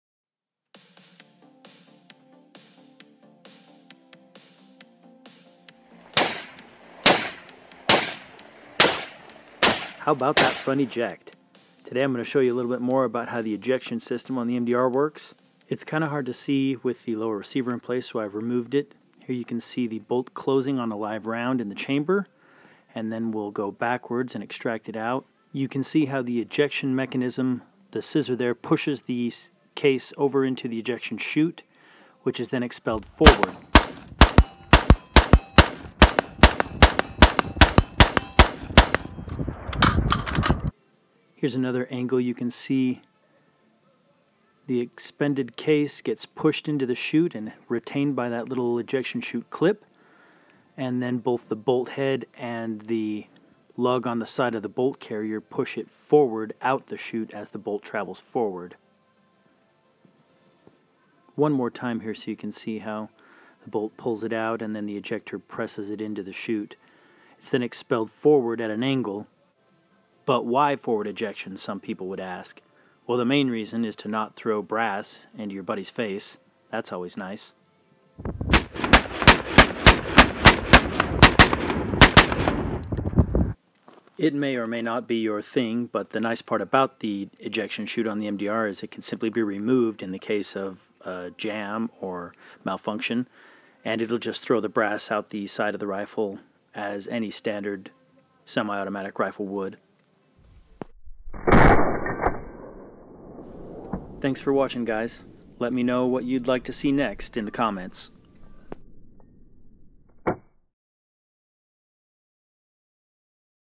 Desert Tech MDR Ejection
An up close look at how the ejection system on the MDR works.